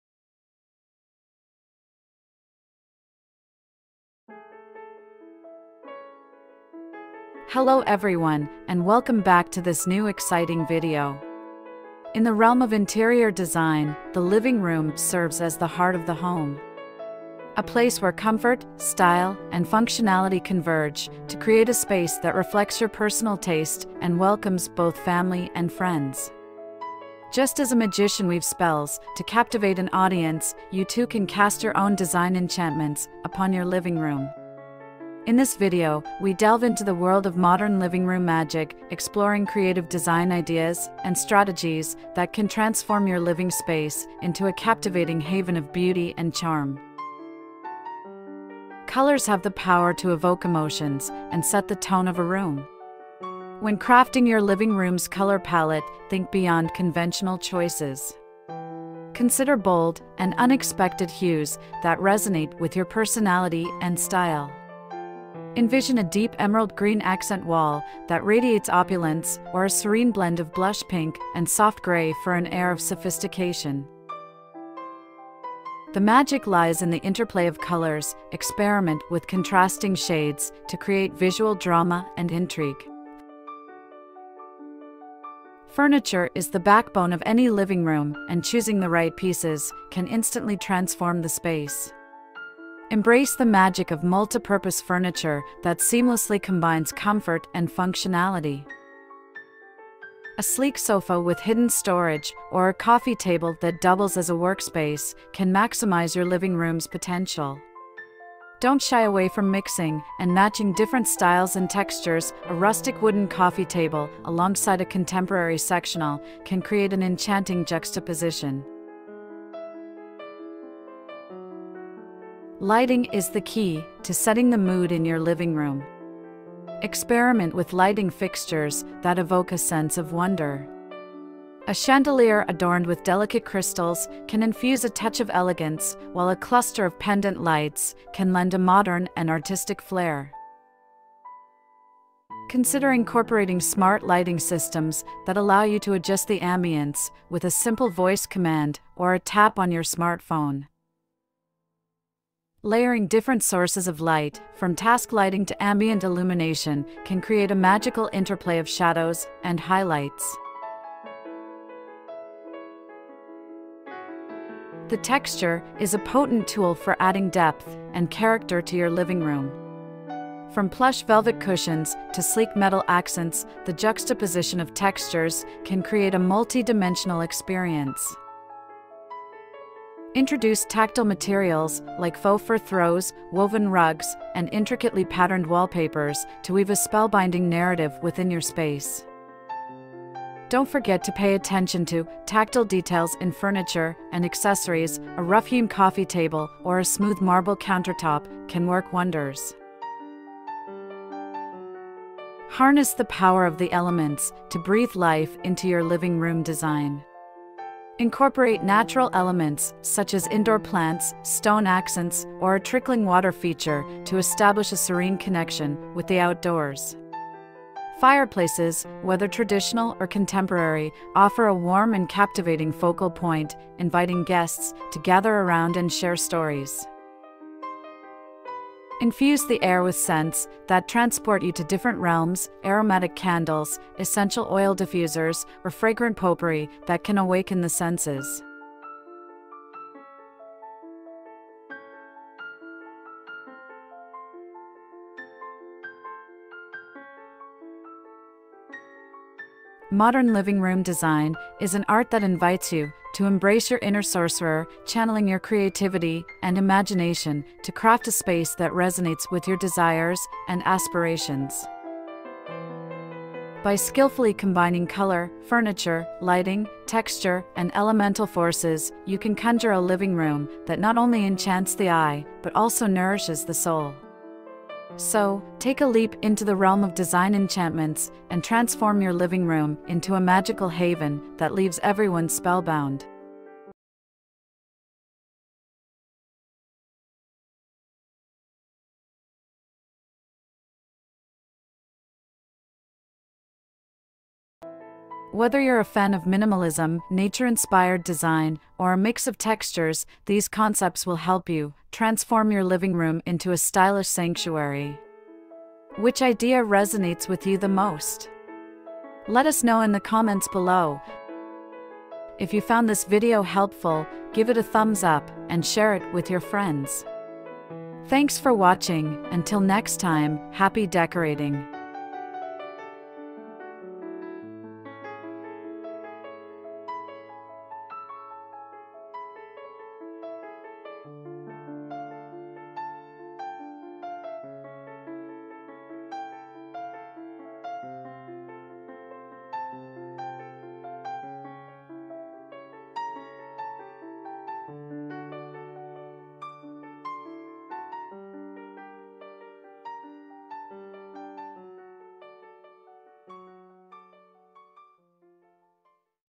Modern Living Room Magic: Transform Your Home with These Design Enchantments: Living Room Decor
In this video, we unveil the secrets of Modern Living Room Magic, guiding you through an enchanting journey that will empower you to transform your living space into a contemporary masterpiece.

Unleash Your Creativity: Discover how to infuse your living room with a touch of magic that reflects your unique style and personality. Our expert tips and insights will help you explore innovative design ideas and bring your creative visions to life.

Elevate with Elegance: Learn how to create an ambiance of sophistication and elegance that elevates your living room from ordinary to extraordinary. Discover the art of choosing the right color palettes, furniture pieces, and decorative accents that exude modern charm.

Enchanting Minimalism: Dive into the world of minimalistic design that maximizes space and fosters a sense of serenity. Explore how to achieve a clutter-free living room while maintaining a cozy, inviting atmosphere.

Tech-Savvy Wonders: Immerse yourself in the realm of technological marvels that seamlessly integrate with your modern living room. Uncover the latest trends in smart home automation, entertainment systems, and lighting solutions that add an element of enchantment to your space.

Artistry in Design: Embark on a journey of artistic expression as we delve into the realm of living room artistry. Discover how to curate captivating art pieces, sculptures, and decorative elements that serve as focal points and conversation starters.

Whether you're an interior design enthusiast, a homeowner seeking a captivating living room makeover, or simply someone who appreciates the allure of modern design, this video is a must-watch. Join us as we unravel the mysteries of Modern Living Room Magic and empower you to create a living space that mesmerizes and enchants.

Also, you can watch
Links :